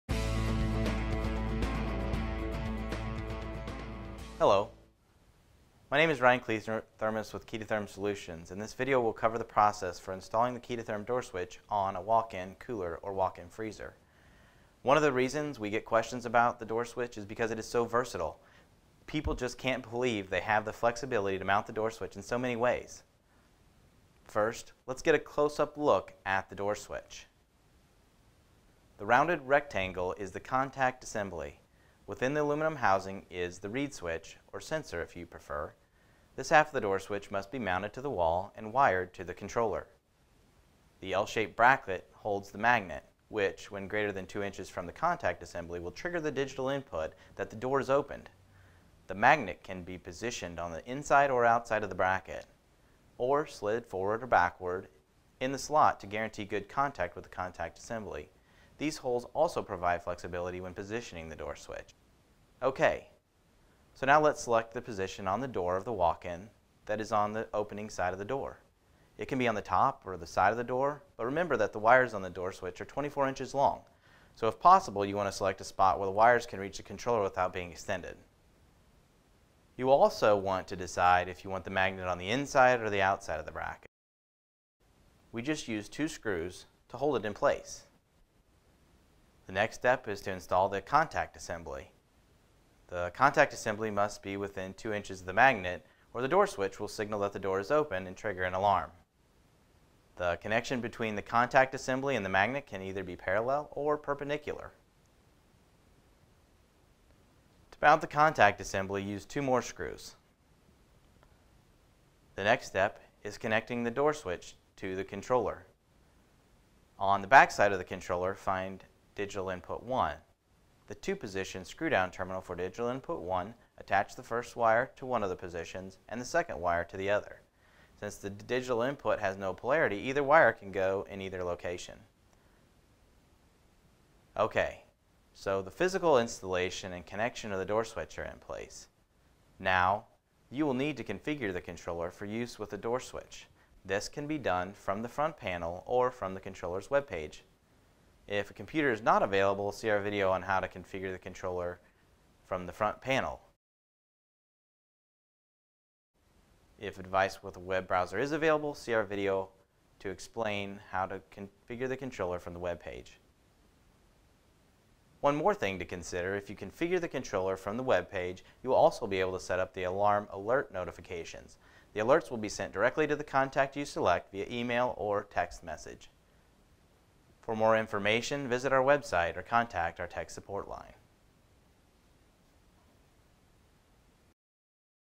How to Install and Wire a KE2 Door Switch - Video 038 (FAQ: Walkin Coolers, Walk in Freezers)
Hello my name is Ryan Kliethermes with KE2 Therm Solutions. In this video I’ll cover the process for installing the KE2 Therm door switch on a walk-in.

One of the reasons we get questions on the door switch is because it is SO versatile. People just can’t believe they have the flexibility to mount the door switch in so many ways.

First lets get a close up look at the door switch.

The rounded, rectangle is the Contact Assembly. Within the aluminum housing is the reed switch, or sensor, if you prefer.

This half of the door switch must be mounted to the wall, and wired to the controller.

The L shaped bracket holds the magnet, which, when greater than 2” from the contact assembly will trigger the Digital Input, that the door is opened.

The magnet can positioned on the inside or outside of the bracket,

or slid forward or back in the slot, to guarantee good contact with the Contact Assembly. These holes, also provide flexibility when positioning the door switch.

Okay, so now let’s select a position on the door of the walk-in that is on the door opening side

it can be on the top, or side of the door.

But, remember, the wires on the door switch are 24 inches long, so if possible, you want to select a spot where the wires can reach the controller, without being extended.

You will also want to decide if you want the magnet on the inside, or the outside of the L-bracket.

In this example, we will use an inside mount, which is how the door switch ships from the factory.

Now, we just use two screws to hold it in place.

The next step is to install the Contact Assembly.

The contact assembly must be within 2 inches of the magnet, or the door switch will signal that the door is open, and trigger an alarm.

The connection between the Contact Assembly and the magnet can be either parallel or perpendicular.

To mount the Contact Assembly, use two more screws.

The next step is connecting the door switch to the controller.

On the KE2 Evap style controller, the pluggable connectors for the Digital inputs are on the left side when looking at the back of the controller.

Find the two position screw down terminal for Digital Input 1. Connect the two wires to the pluggable connector, orientation does not matter

On the KE2 Low Temp or KE2 Adaptive style controller, the screw down terminals for the Digital inputs are on the back of the display cover.

Choose one of the two available Auxiliary inputs. These inputs are on the right side when looking at the back of the display cover.

Connect the two wires to the screw down terminal, orientation does not matter

Okay, so the physical installation and connection of the door switch are in place.

Now you will need to configure the controller for use with a door switch. This can be done from the front panel or from the controller’s web page.

Video 40 explains how to configure the controller from the front panel, and Video 041 explains how to configure the controller from the webpage.

One more thing I want to mention, if you configure the controller from the webpage, you will also be able to setup alarm alert notifications. The alerts will be sent directly to the contact you select, via text or e-mail.